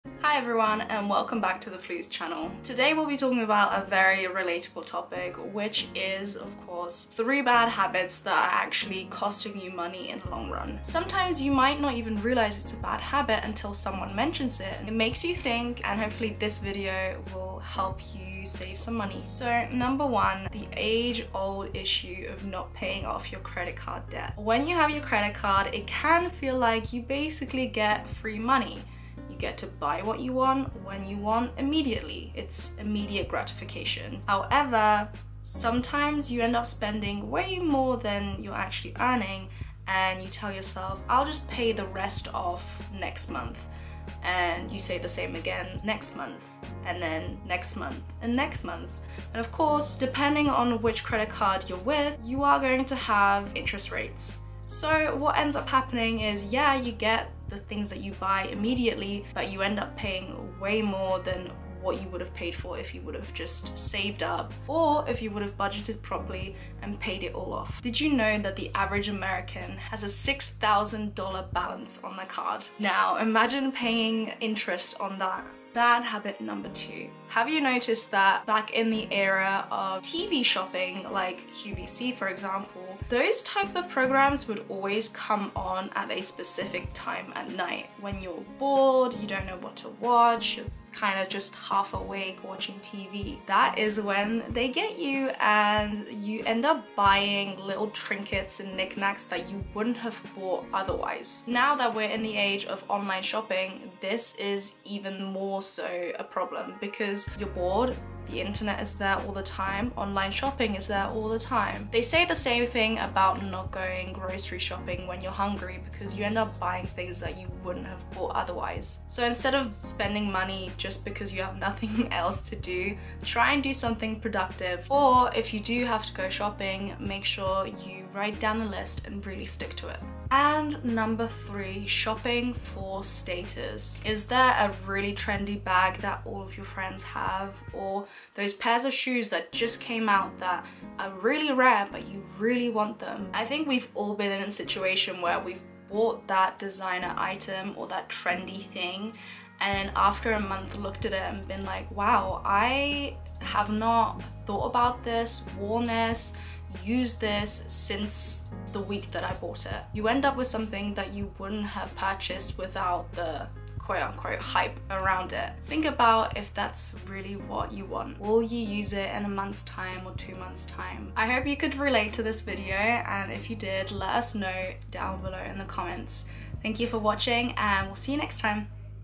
3 Bad Spending Habits Costing You Money In The Long Run | Fluzapp
1. Not paying off credit card debt and high interest rates
2. Shopping while bored and/or hungry
3. Shopping for status